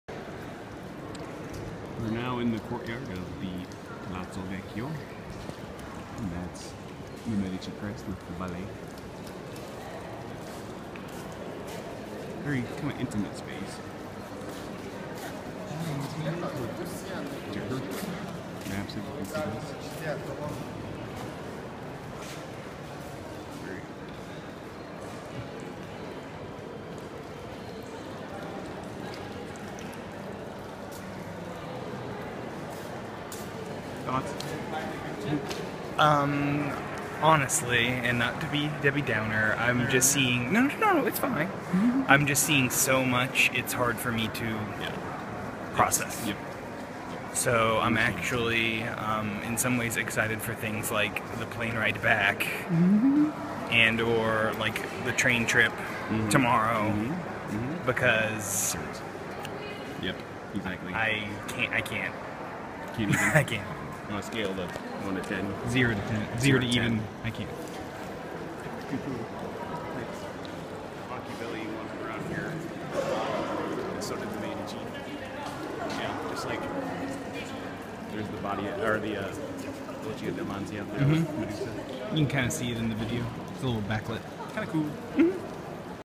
2015.06.06 - Firenze: Courtyard of the Palazzo Vecchio
Wherein we enter one of my favorite buildings ever--the heart of historically republican Florence